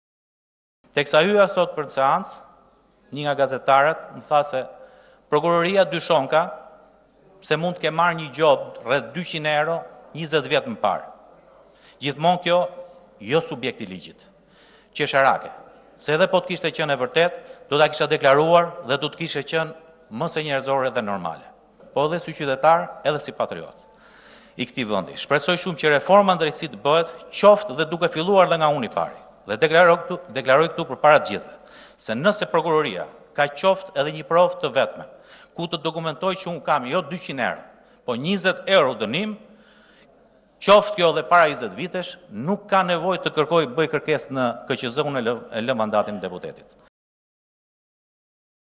Report TV - Artan Gaçi: Më hetoni! Nëse kam 20 euro gjobë e lë mandatin që sot
TIRANE-Deputeti i Partisë Socialiste, Artan Gaçi gjatë fjalimit të tij në Kuvend i ka bërë thirrje Prokurorisë së Përgjithshme që të nisë menjëherë verifikimet për personin e tij lidhur me deklalarat se mbi të rëndon një gjobë prej 200 eurosh.

Gaçi tha se nëse provohet se ai ka një gjobë qoftë edhe 20 euro, do ta lërë menjëherë mandatin.

“Ndaj Artan Gaçit nuk ka asnjë gjobë jo 200 euro, por as 20 euro nuk ka. Këtu ka diçka, nuk është thjeshtë një lajthitje. Kam hyrë i ndershëm në politikë dhe me shumë biznese ndërkohë që vazhdoj të jem i ndershëm dhe me më pak biznese. Përfshirja ime në atë listë është dashakeqe dhe më së shumti një lojë e shëmtuar politike për të hequr vëmendjen nga reforma në drejtësi”, u shpreh Gaçi.